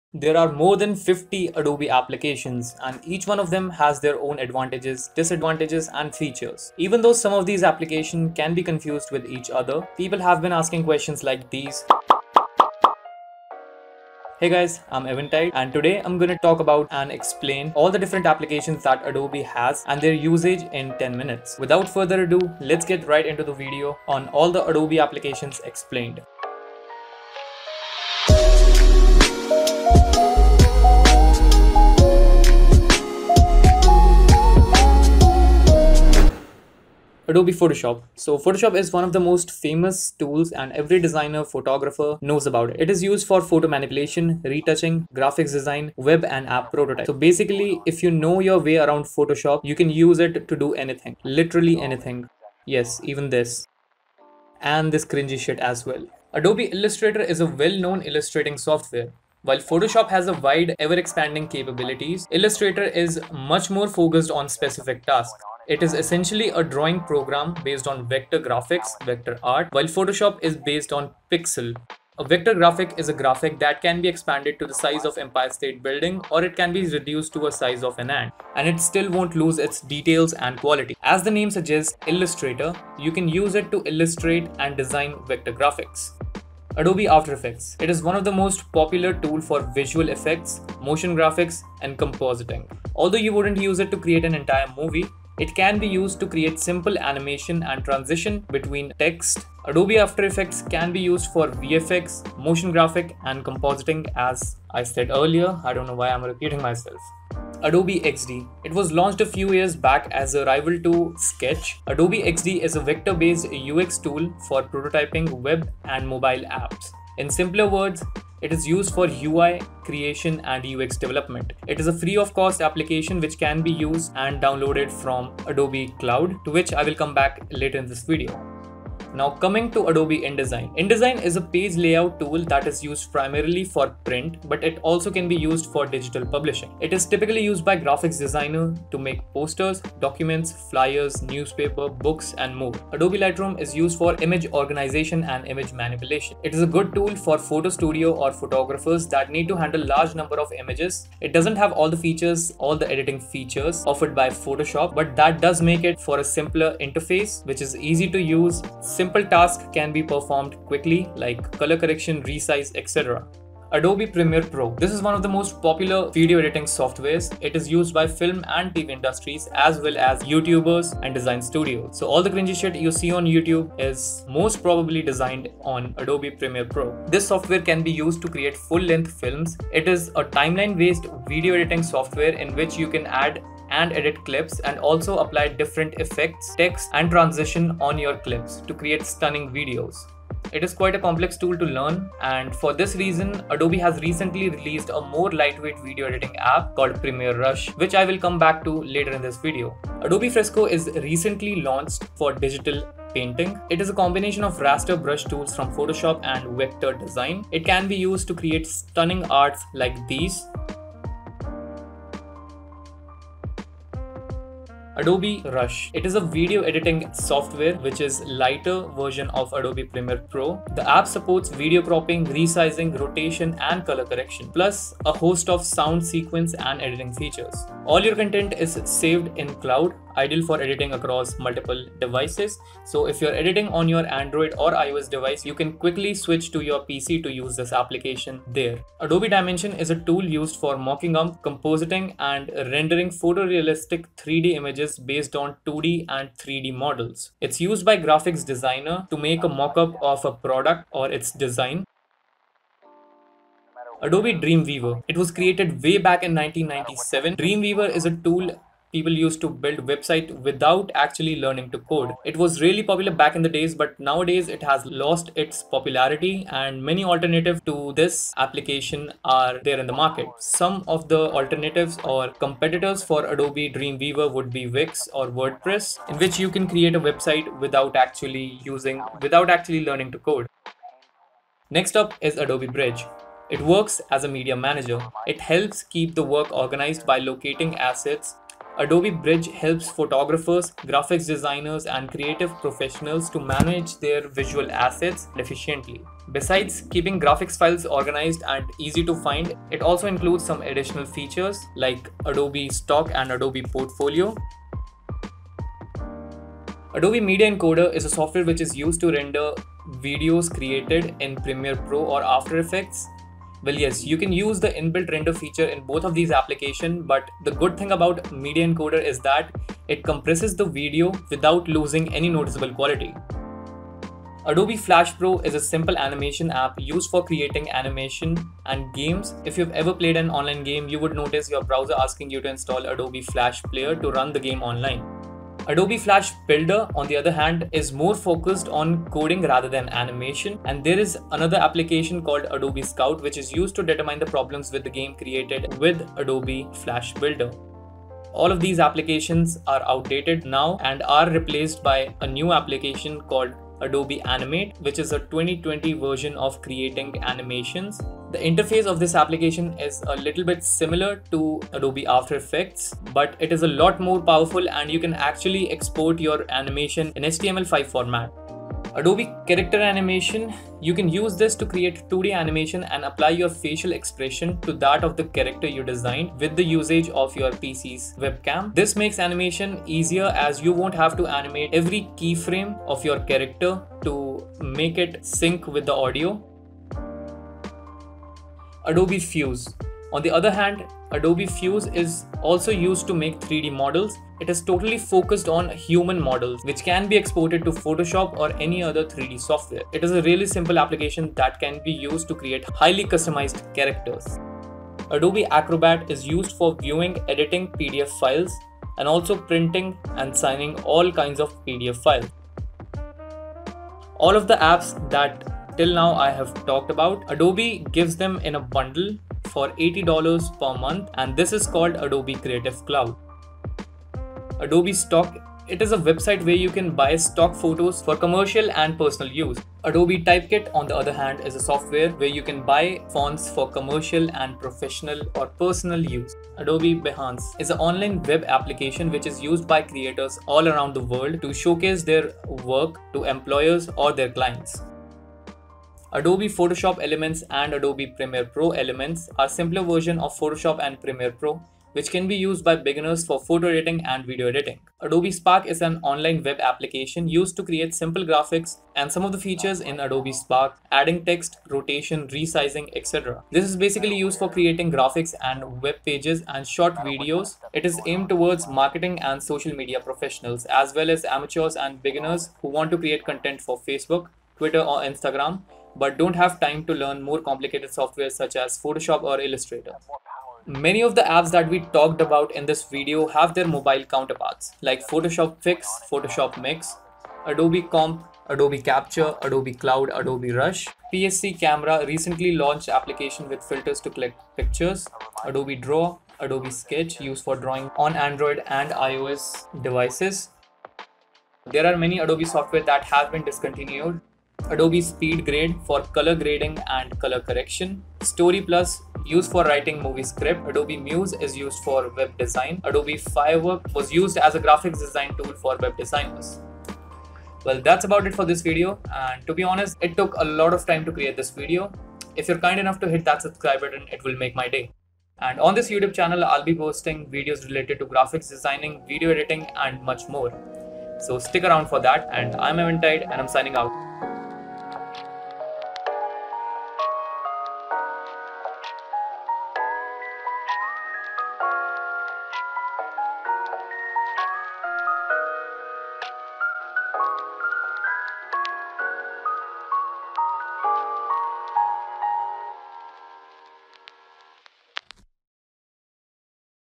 All Adobe apps explained under 10 minutes
Hey guys, by the title All Adobe apps explained under 10 minutes it's pretty clear what this video is about. In this video, I'll be explaining to you the uses of all the Adobe applications, well almost all the adobe apps.
Many people who are new to this have confusing questions regarding similar apps like "what is difference between after effects and premiere pro" or "What is the difference between Photoshop, Illustrator, Lightroom, and Bridge".
So, I made this video to quickly give you an overview of all adobe products.